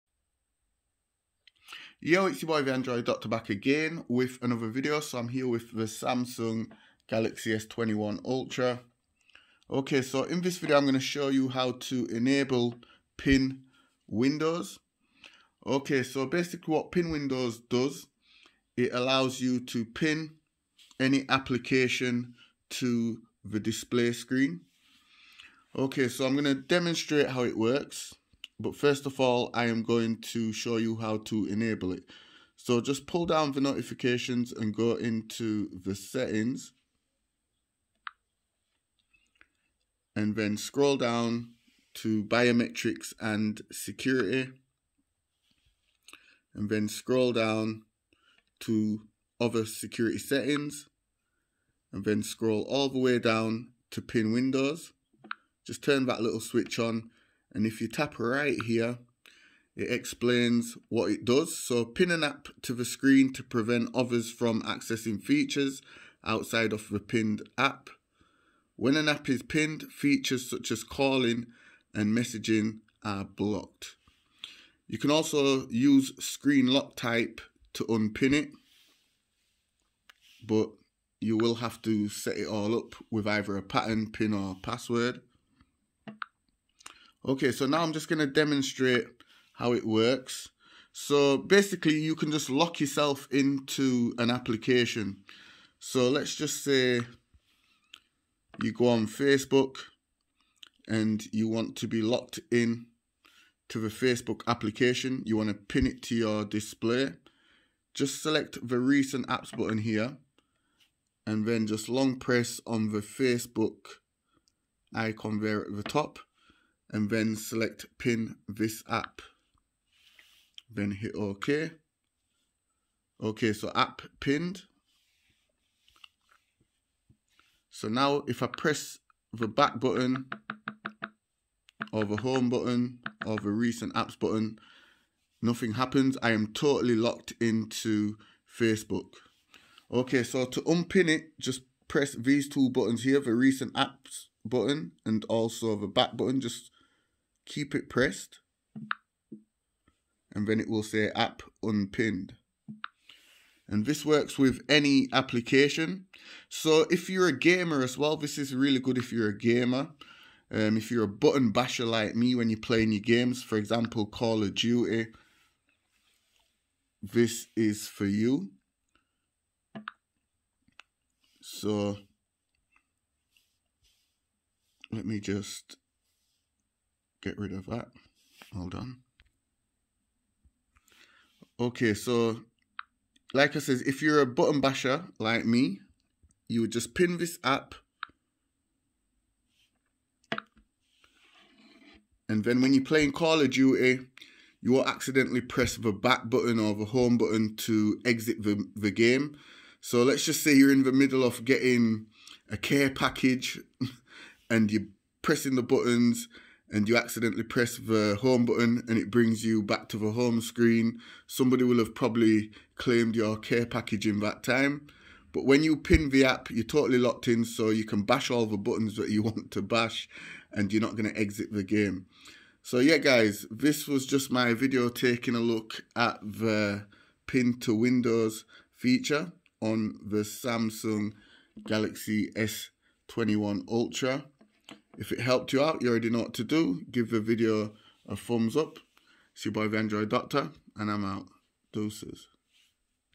Samsung Galaxy S21 Ultra Screen Pinning Pin Windows Very Useful Feature
hi android doctor here again with another video and today i will be showing you how to use the screen pinning feature!!
You can pin an application to the screen of your device. This feature locks your device so the person using it has access to only the pinned application